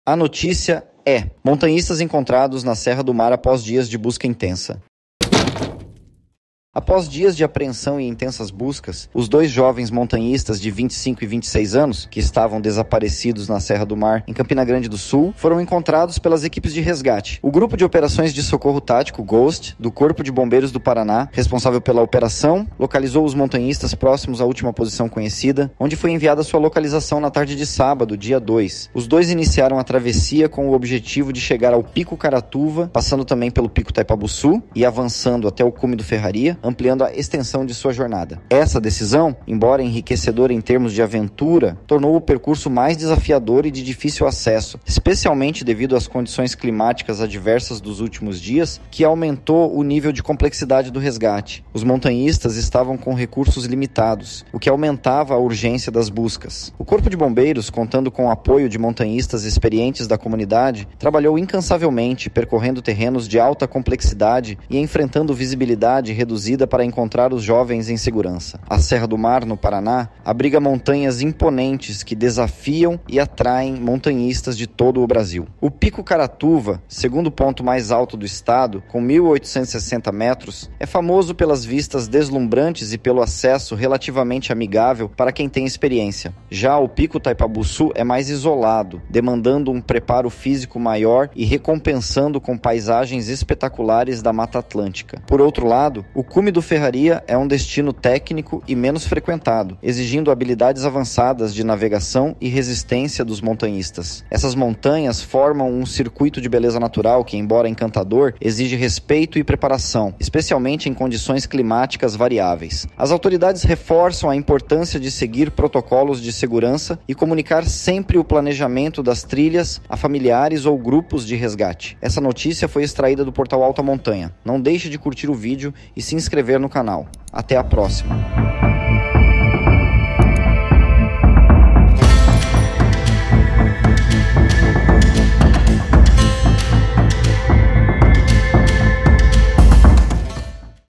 Montanhistas Encontrados na Serra do Mar Após Dias de Busca Intensa
Após dias de apreensão e intensas buscas, os dois jovens montanhistas de 25 e 26 anos, que estavam desaparecidos na Serra do Mar, em Campina Grande do Sul, foram encontrados pelas equipes de resgate..

Saiba Mais!